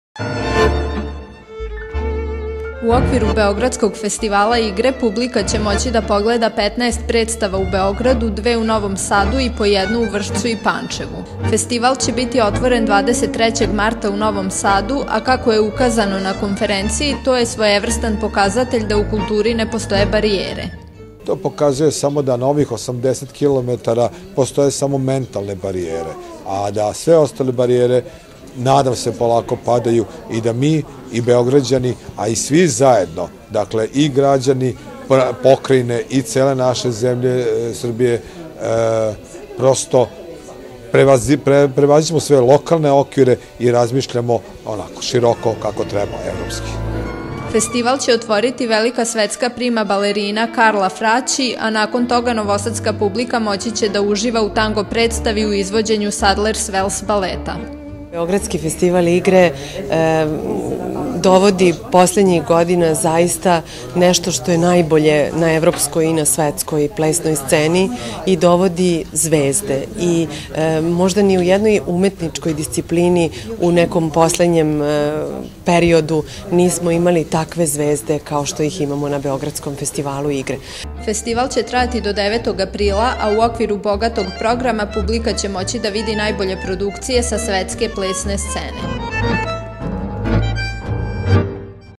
TV KANAL 9, NOVI SAD: "Uhvati svetlost" u Novom Sadu
12. Beogradski festival igre biće održan od 23. marta do 9. aprila, a publika iz Beograda, Novog Sada, Vršca i Pančeva imaće priliku da uživa u nastupima velikih svetskih zvezda.